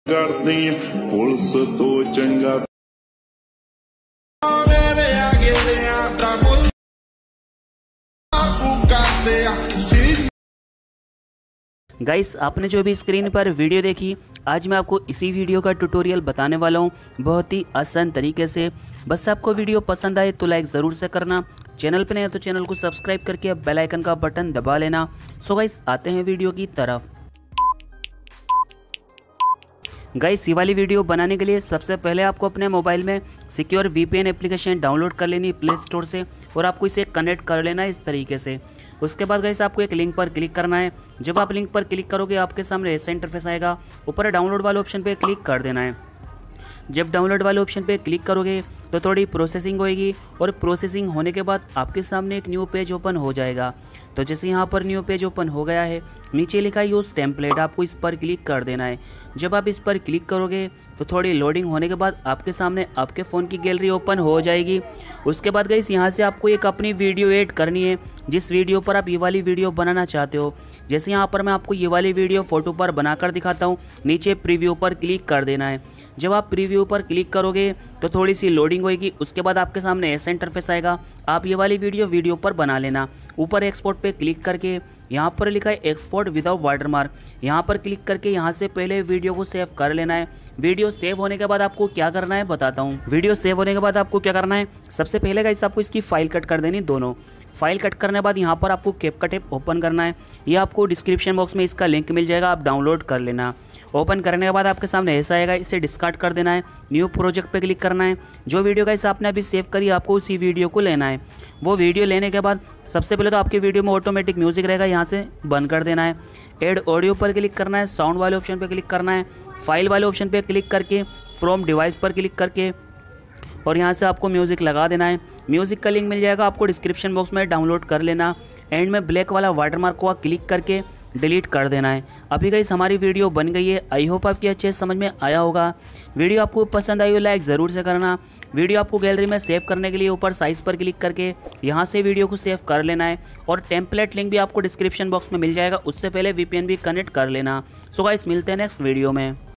sidhu moose wala song reels editing / 1 click me trending reel banaye / capcut template link
🔴 Friends Aap Instagram par Reel Send Karo Aur Banwao Tutorial👇👇-

Music and App download karne se pehle aapke mobile me telegram app install karlena play store se👇👇👇👇👇👇